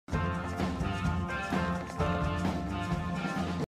The Chase is On! part 2
This map is so cute and funny! Finally just a happy map without any drama, just a backyardigans's song and to kits running from the clan and their clanmates.
Characters: Leafkit, Squirrelkit, Dustpelt (the left tom) and Brambleclaw (the right tom).